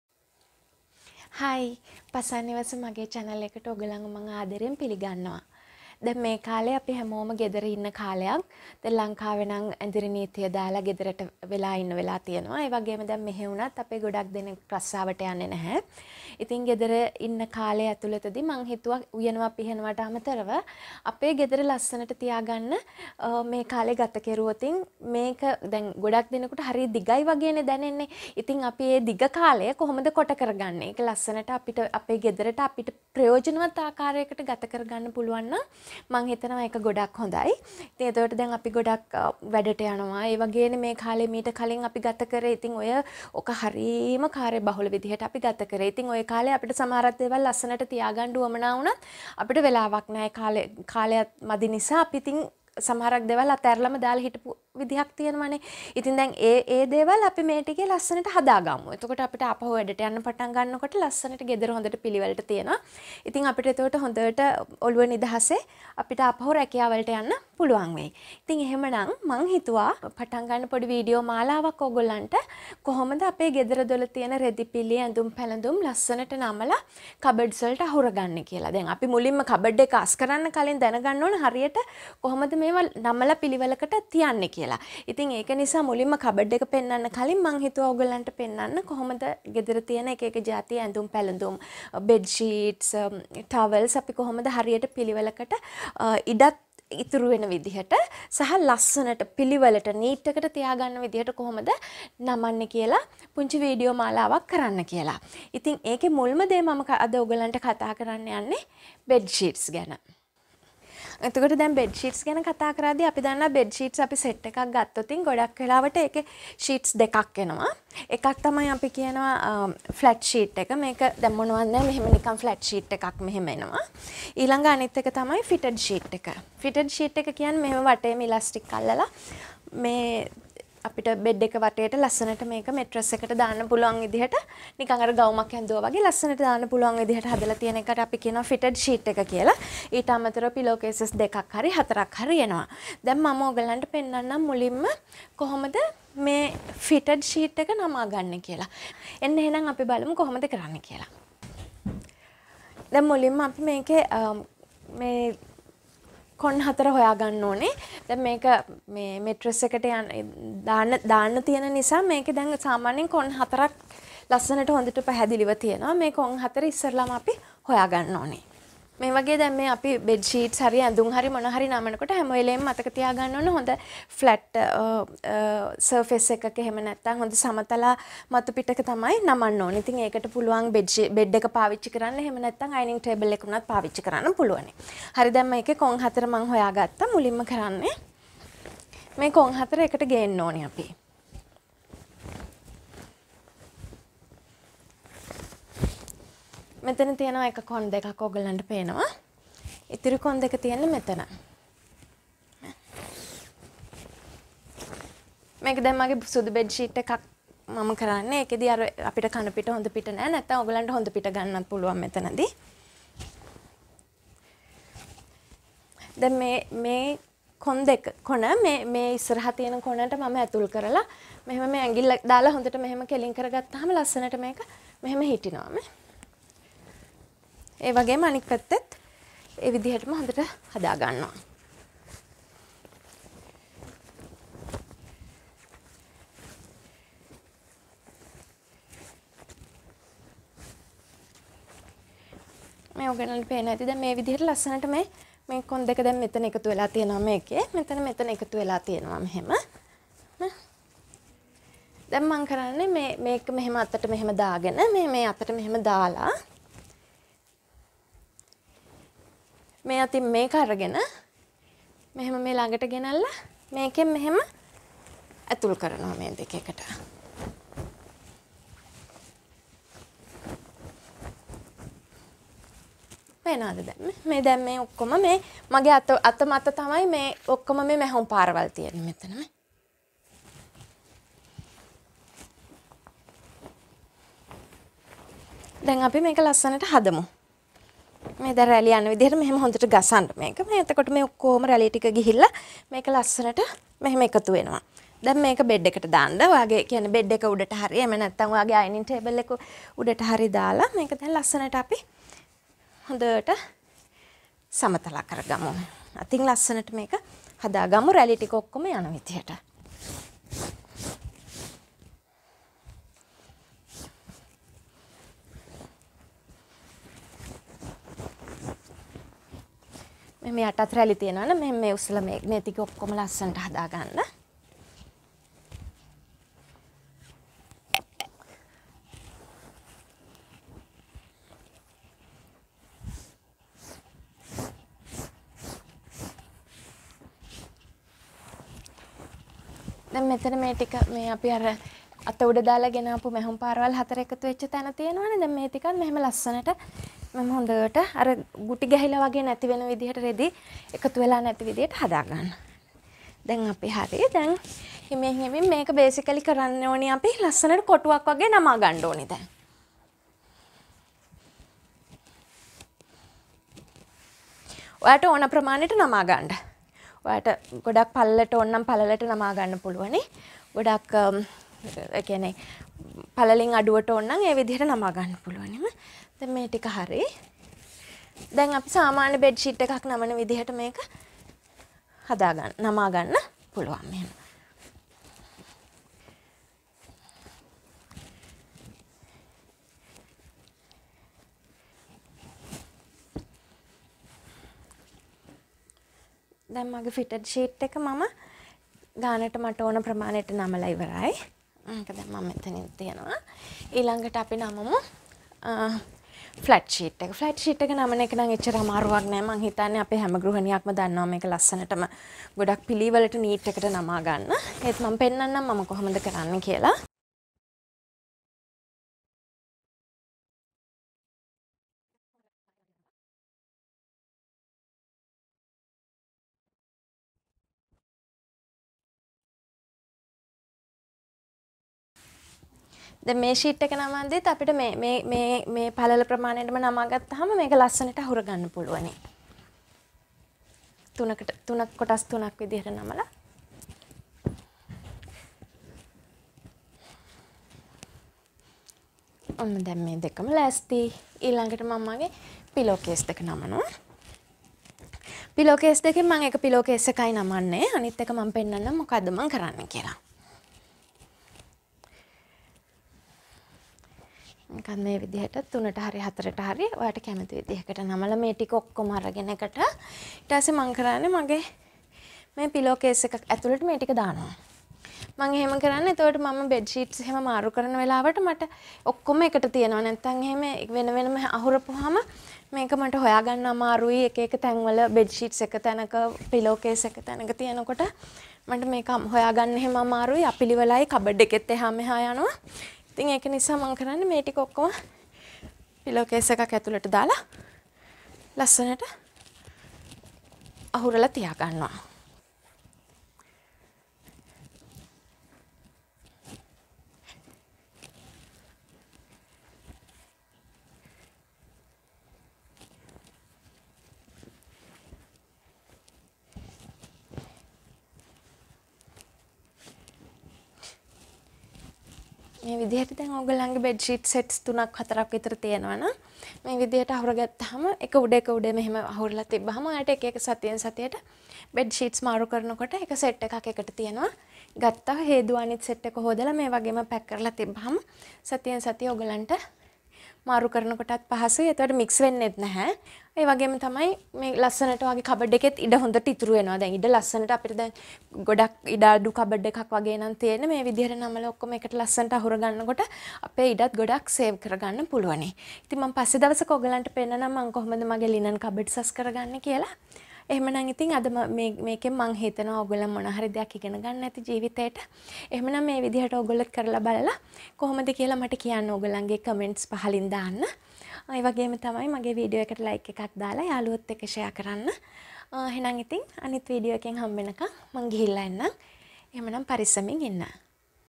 15 - රෙදිපිළි ලස්සනට අහුරමු - පළමු කොටස | Fold Your Bedding Properly - Part 1
First of a series of videos where I will show how to fold and organize clothing...